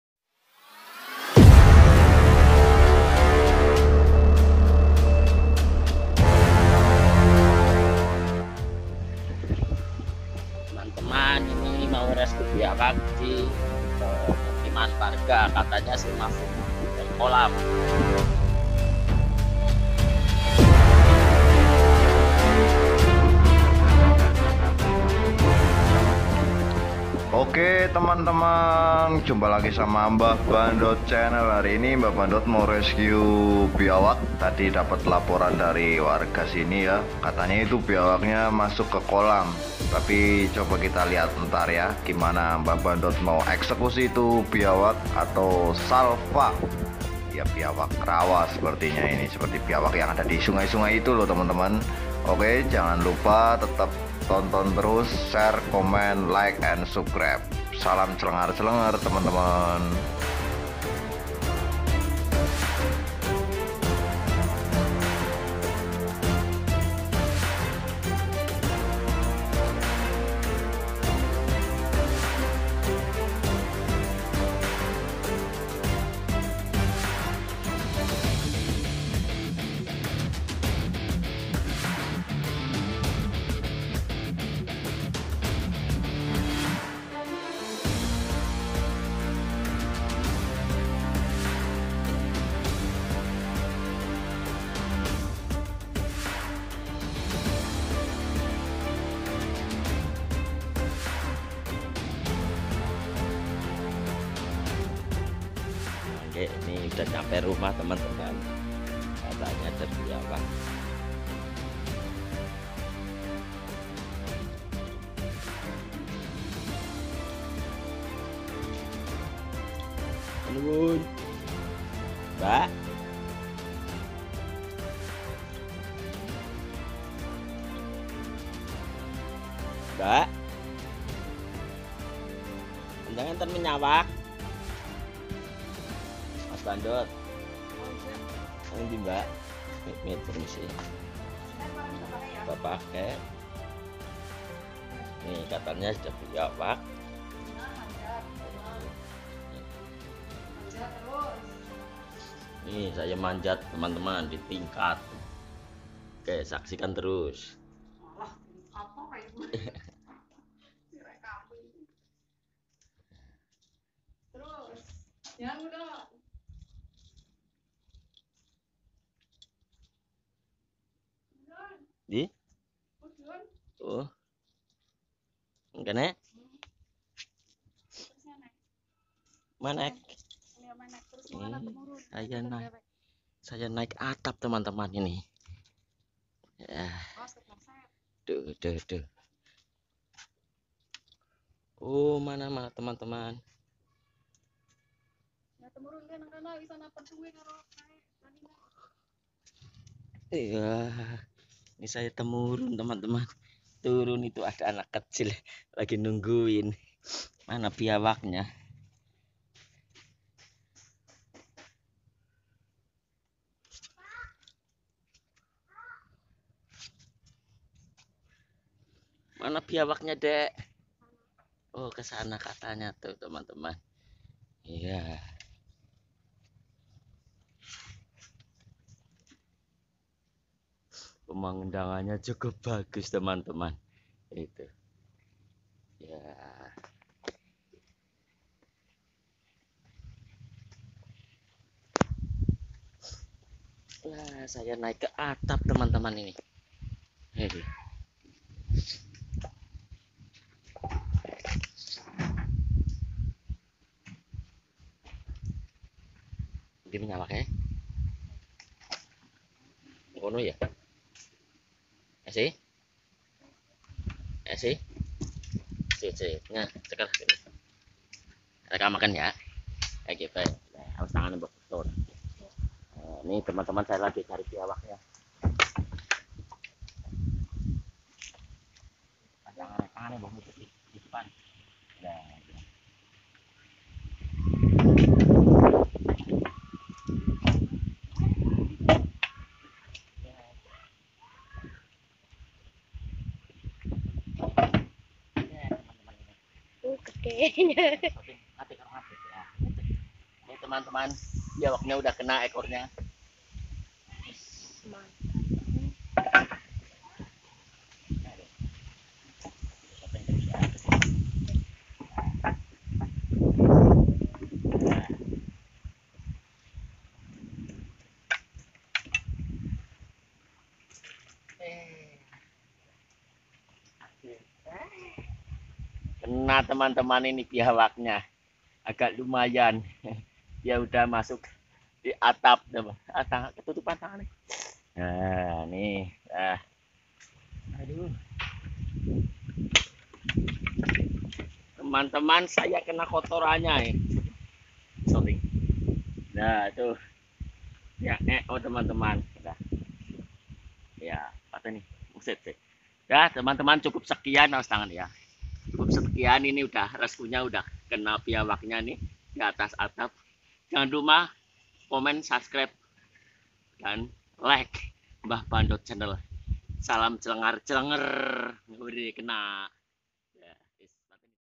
Rescue Biawak Di atap rumah Bu Lina Kedawung
Salvator pemakan daging